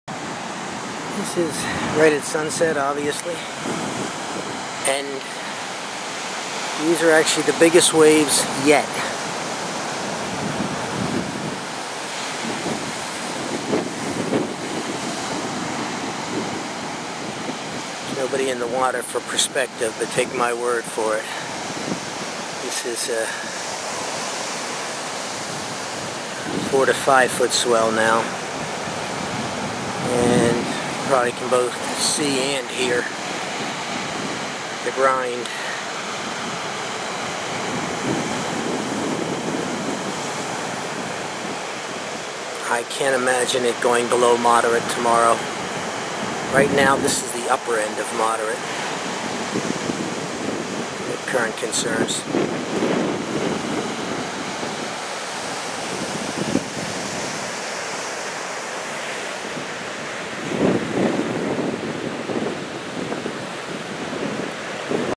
8:30 pm LBI swell has built to 4 to 5 feet
largest waves so far in this recent swell. Very unlikely it will drop overnight.